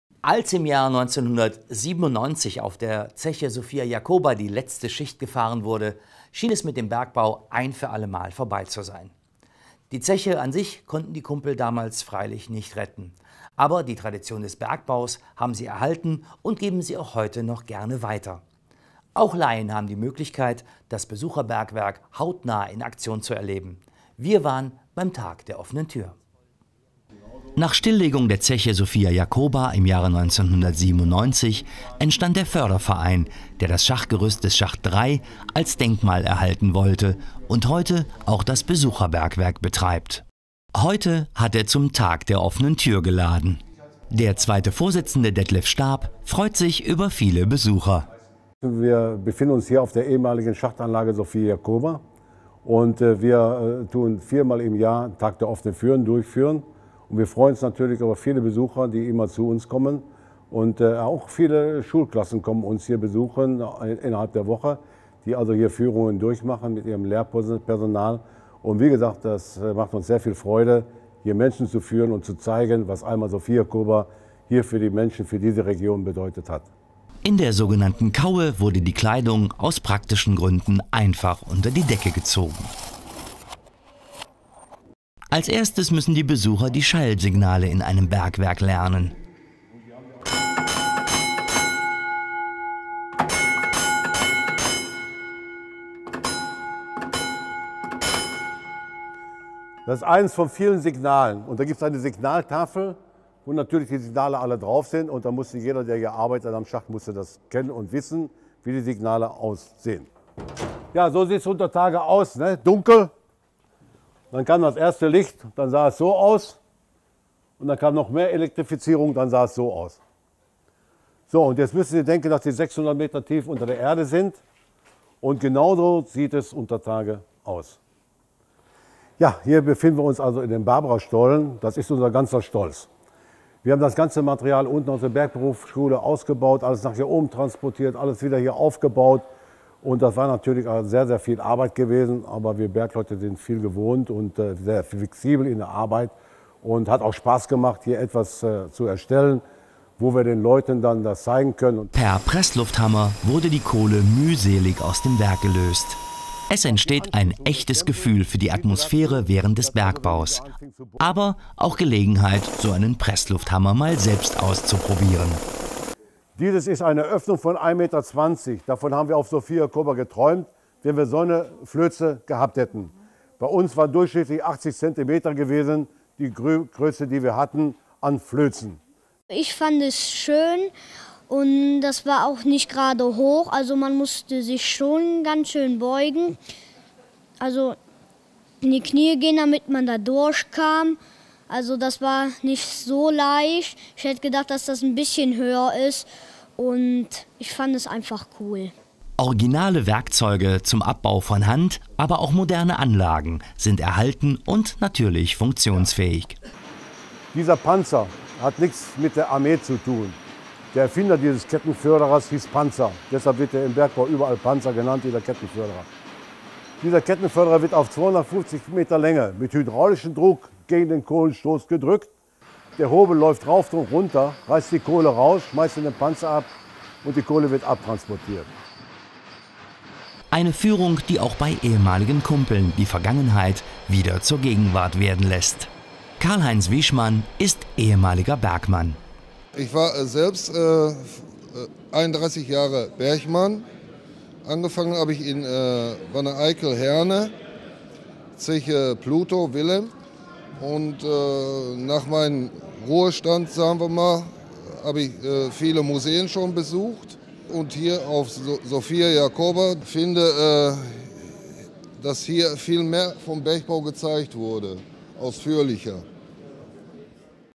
myREGIO.TV - Tag der offenen Tür auf Schacht 3
Als im Jahr 1997 auf der Zeche Sophia-Jacoba die letzte Schicht gefahren wurde,
schien es mit dem Bergbau für allemal vorbei zu sein.
Die Zeche an sich konnten die Kumpel damals freilich nicht retten, aber die Tradition
des Bergbaus haben sie erhalten und geben sie auch heute noch gerne weiter.
Auch Laien haben die Möglichkeit das Besucherbergwerk hautnah in Aktion zu erleben.
Wir waren beim Tag der offenen Tür.